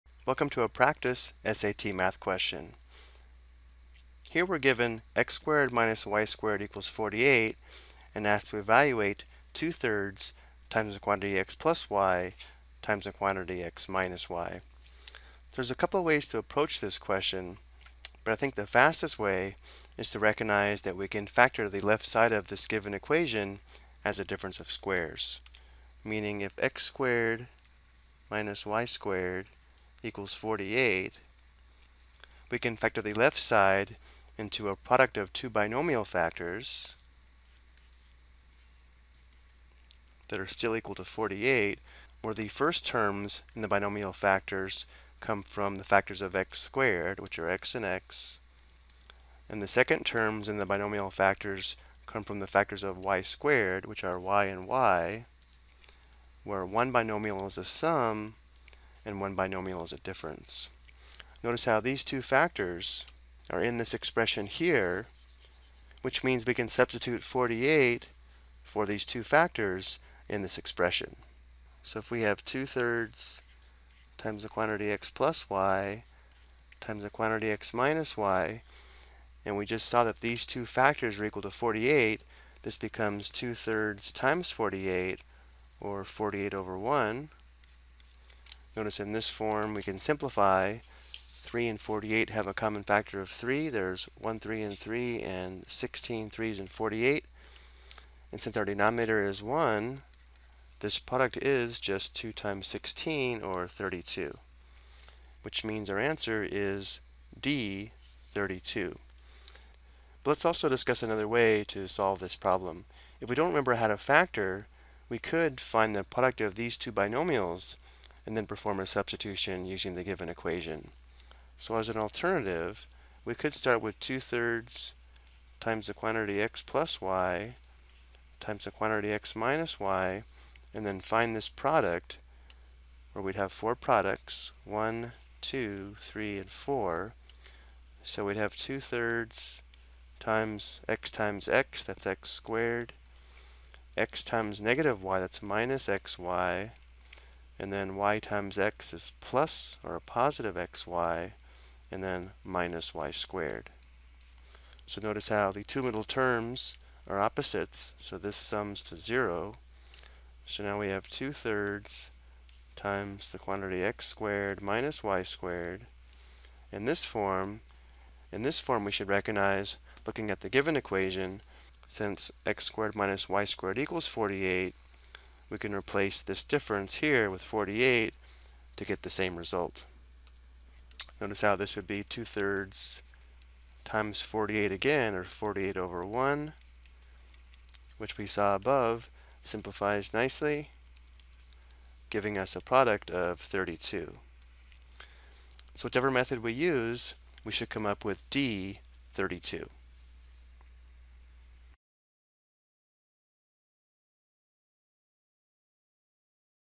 SAT Math (Algebra and Functions) - Practice 2.3
The video provides an explanation of a practice SAT math question involving algebra and functions.